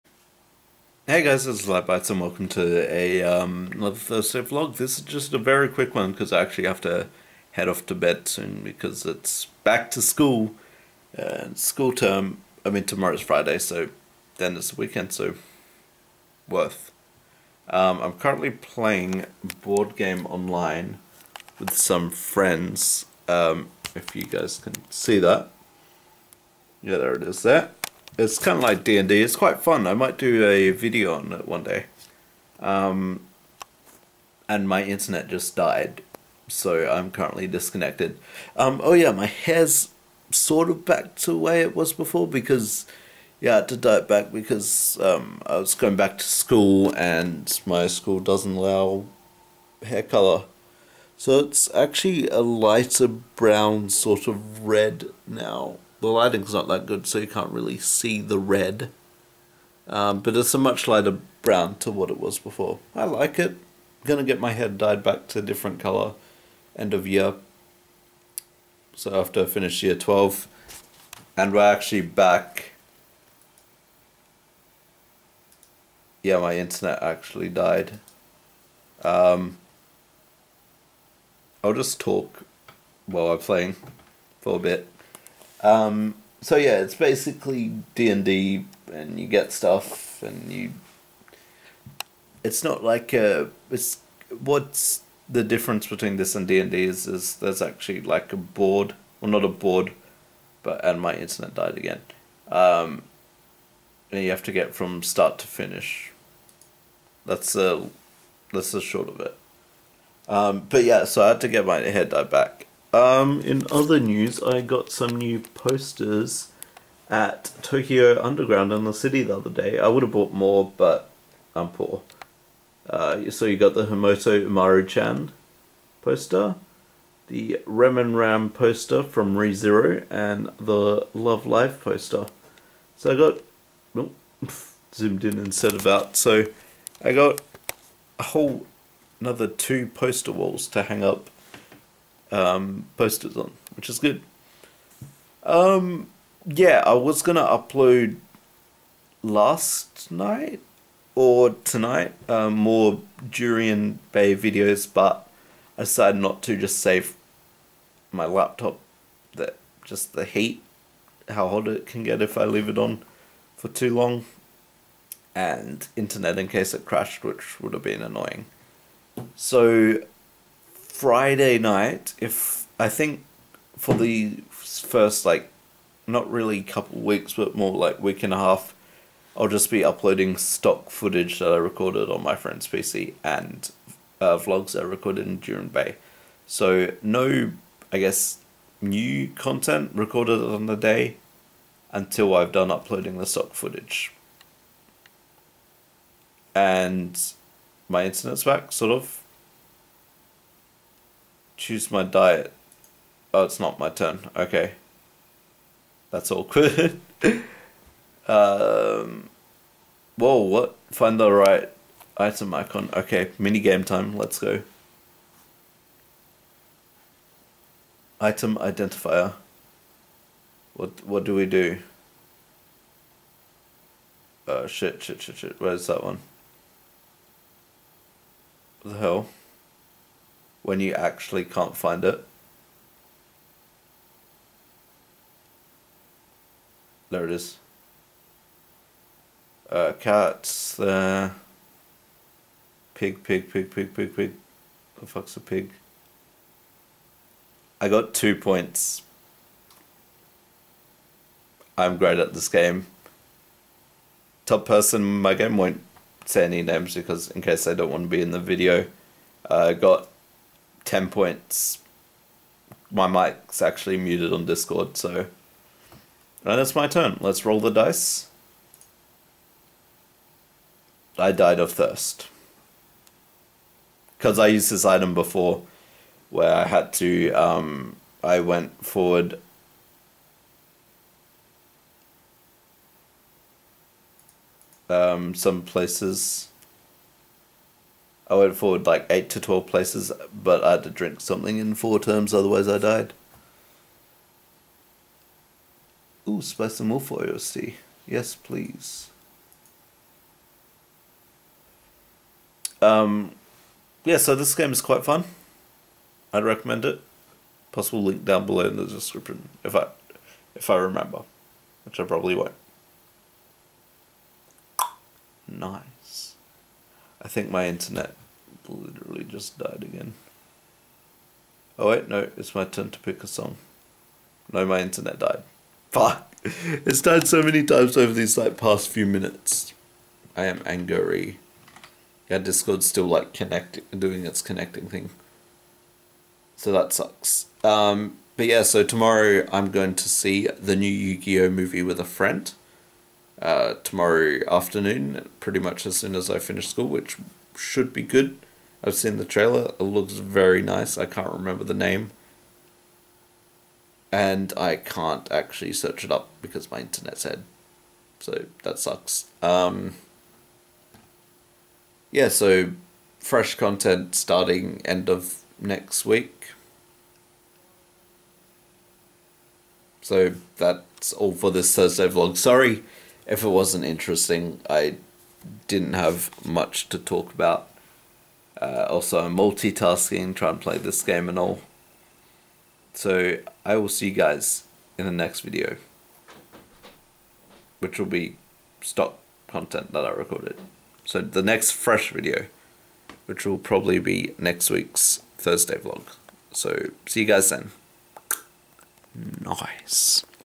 UPDATES & BOARD GAME ONLINE - Thursday Vlog #16
Playin board game online, updates about videos and my internet dying :D